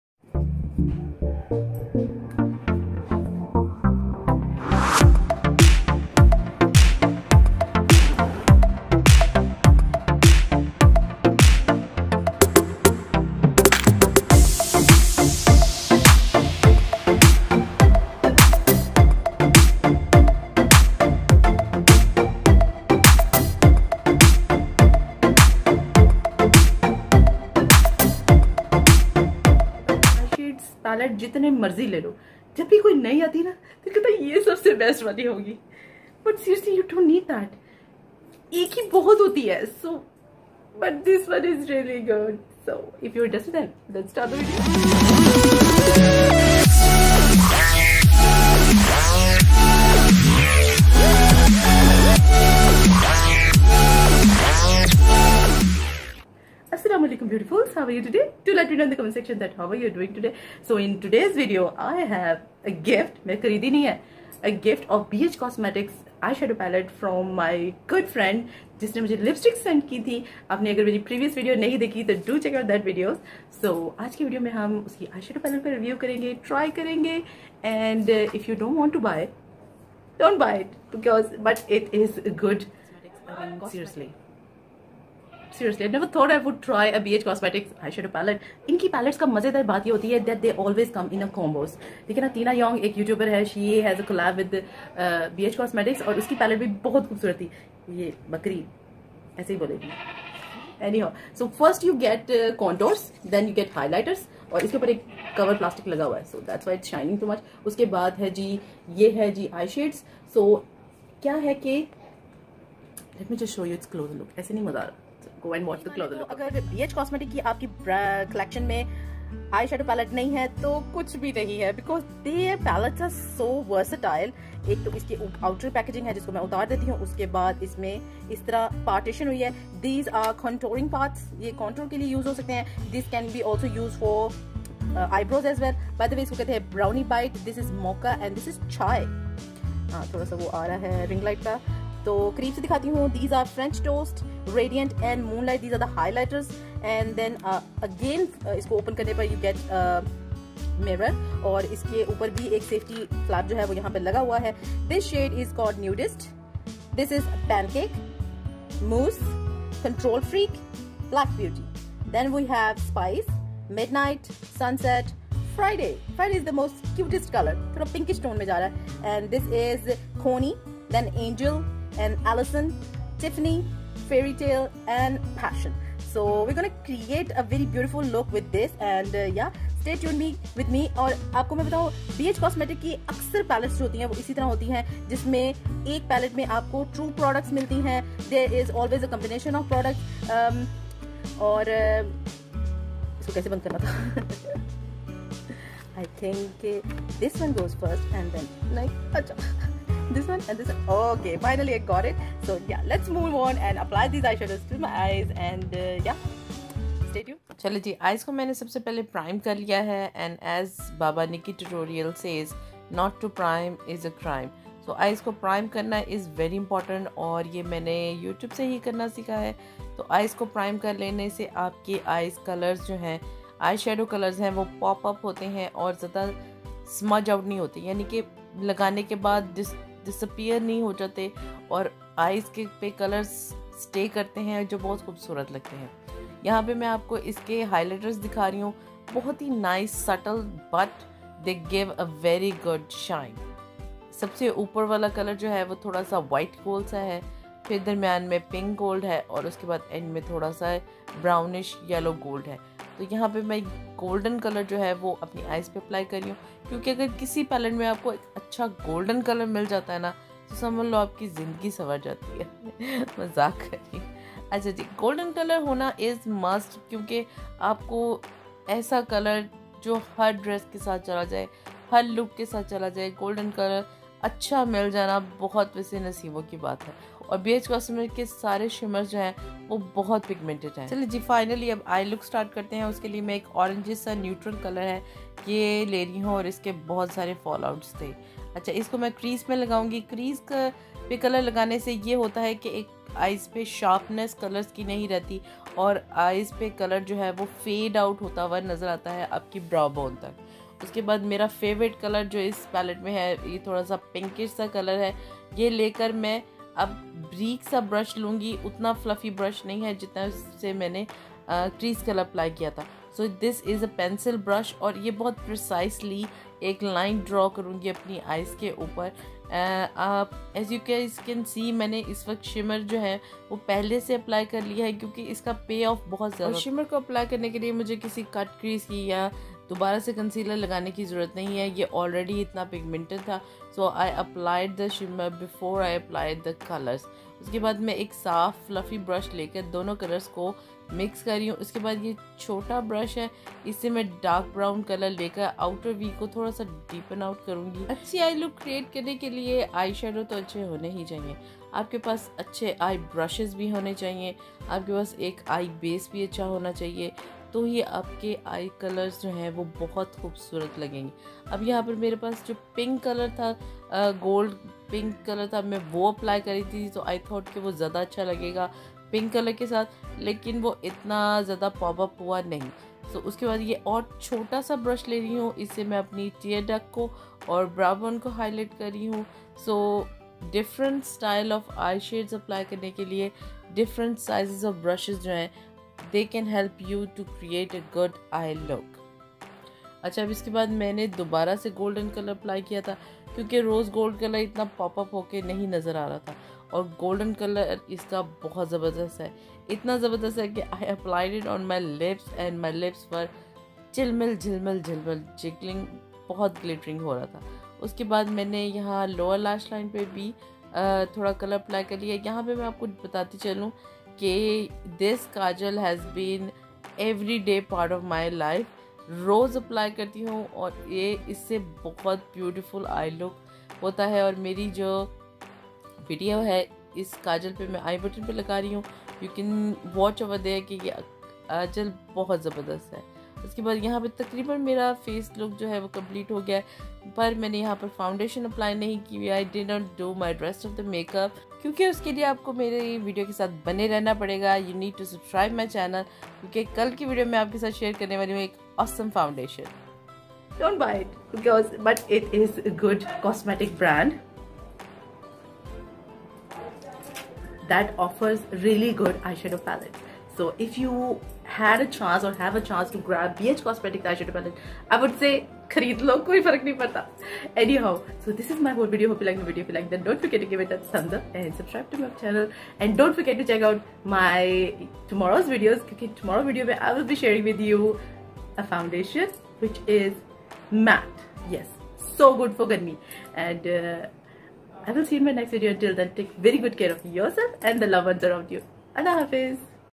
Bh cosmetics eyeshadow palette By beautifully blessed
Hello beautifuls how are you today hope your doing fine and happy with your life.
So on today's video let's discuss bhcosmeric eyeshadow palate is one of the most pigmented palette and I do get it as a gift but its price is over 3 thousand Rs.